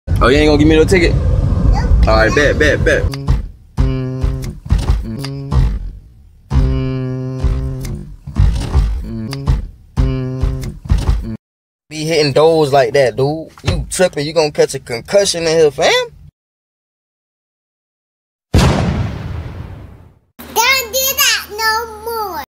In the car with jax 😂(end is funny)
I said dont be jumping..he wanna keep jumping anyway ! Lol

He not hurt tho💯 ENJOY!😎